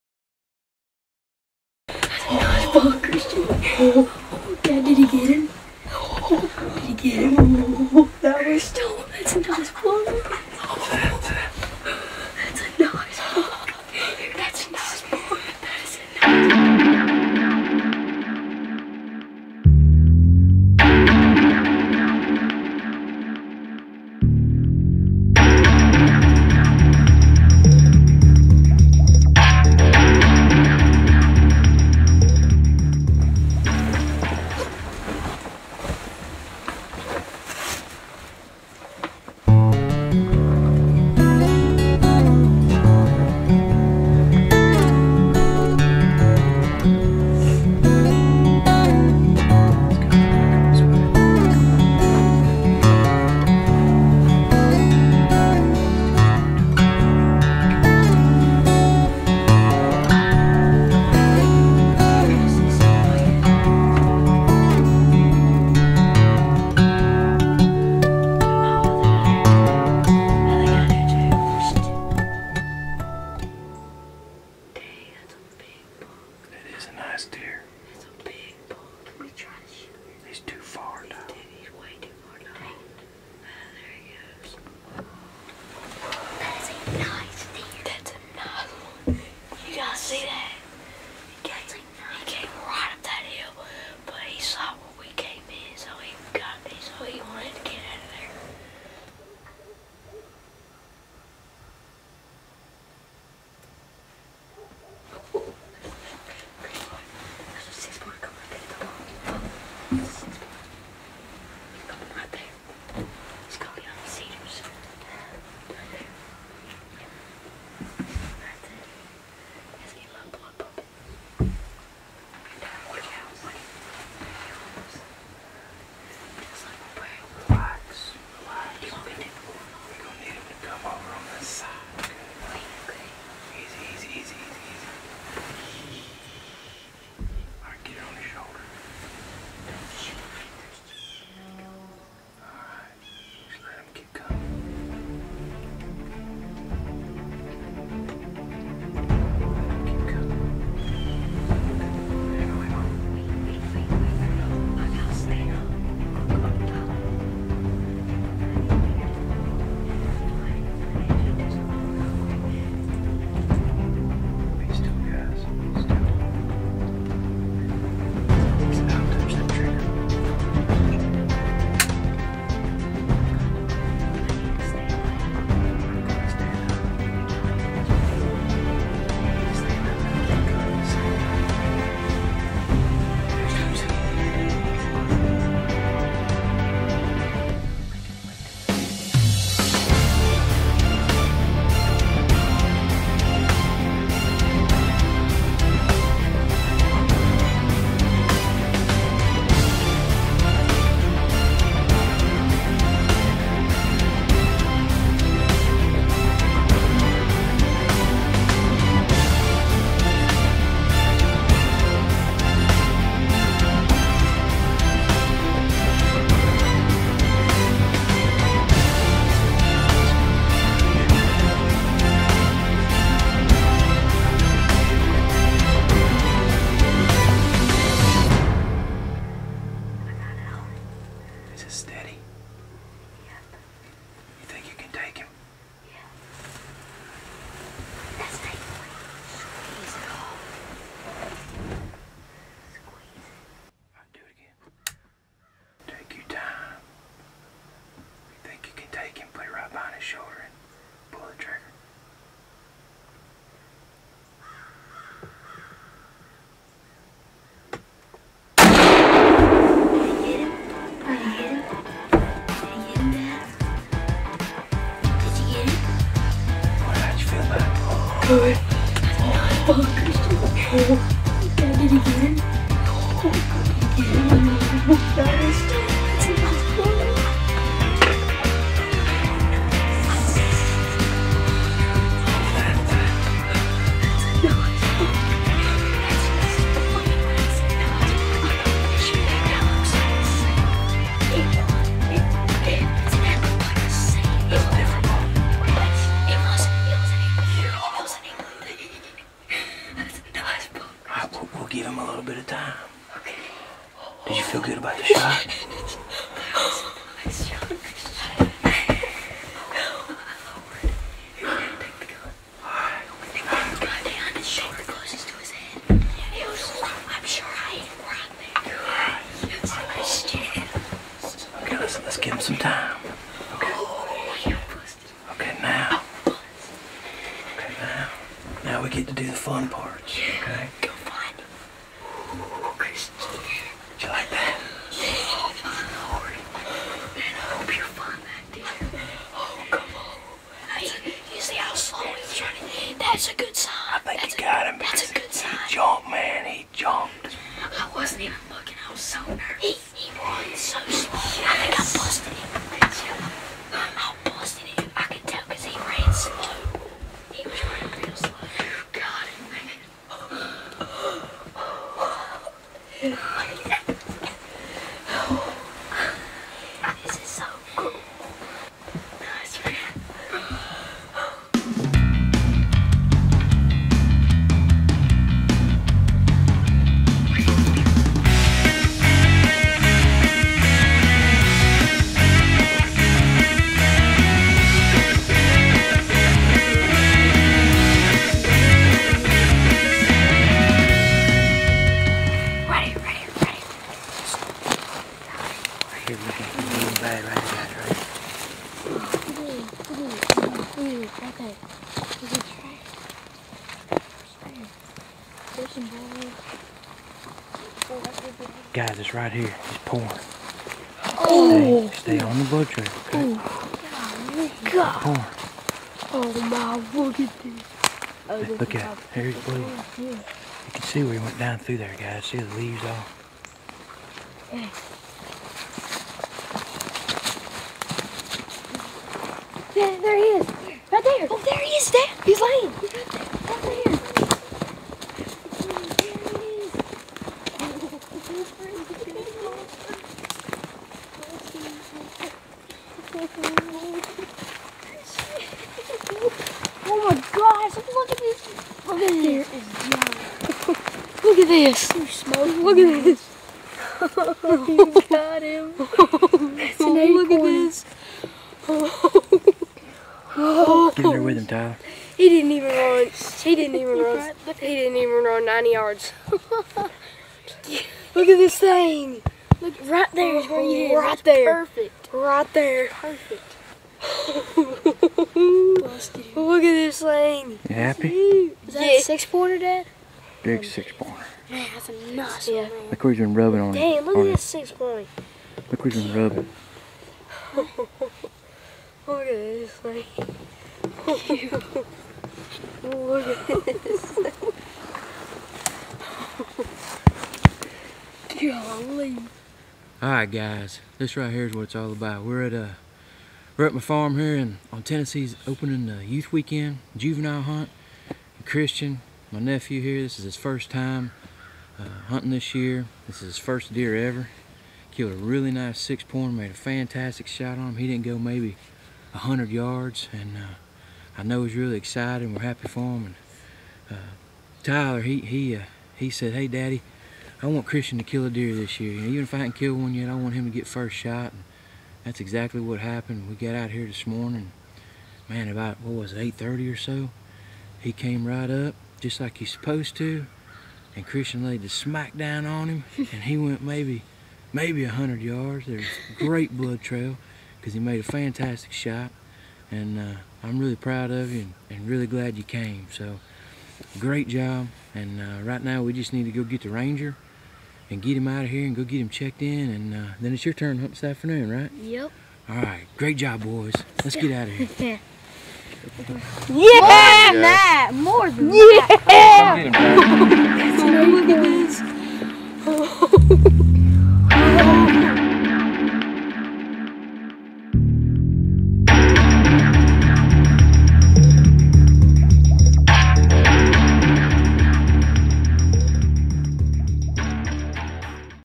Outdoor Addixtions - "Initiation at it's best"
Join us as Craig takes his son and nephew on a deer hunt during the TN youth season.